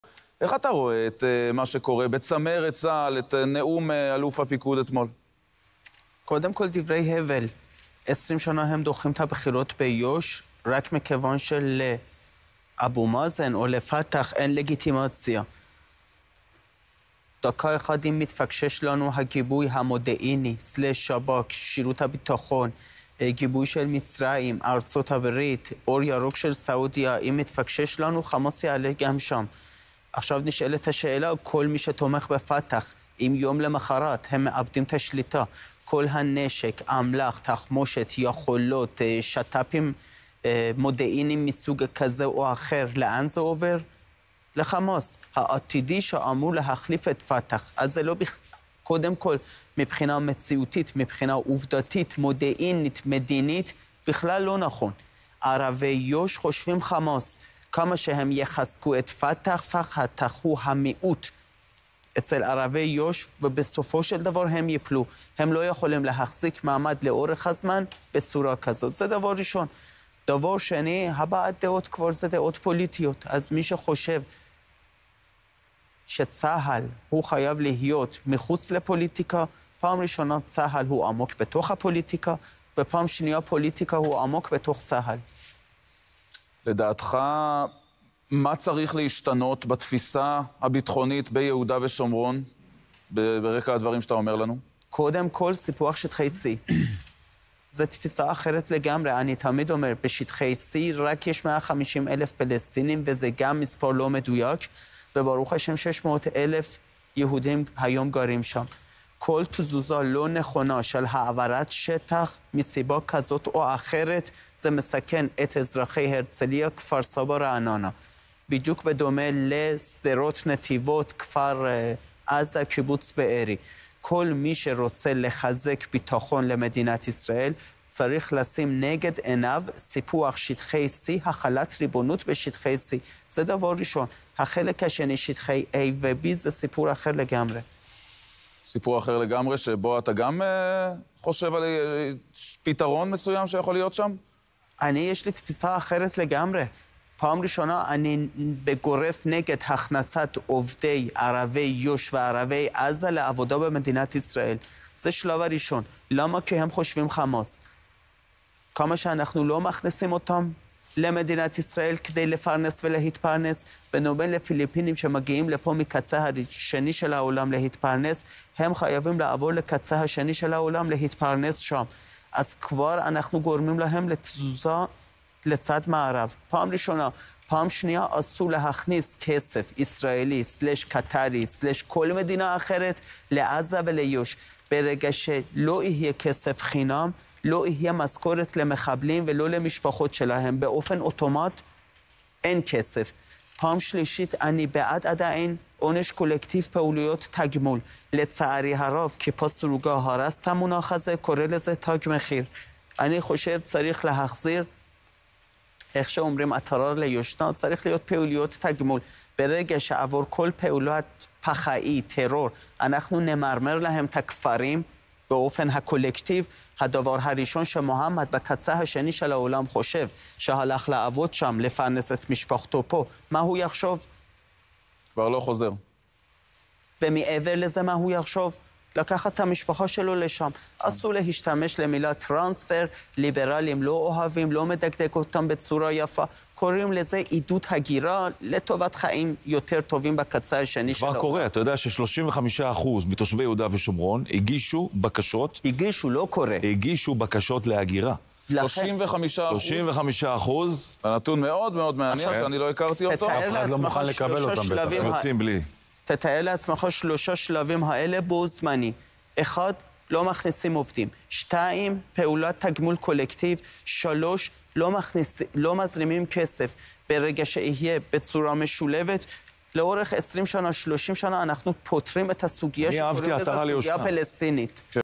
אליהו יוסיאן: "אני נגד הכנסת עובדים ערבים לעבודה בישראל - הם חושבים חמאס"
ערוץארבעשרה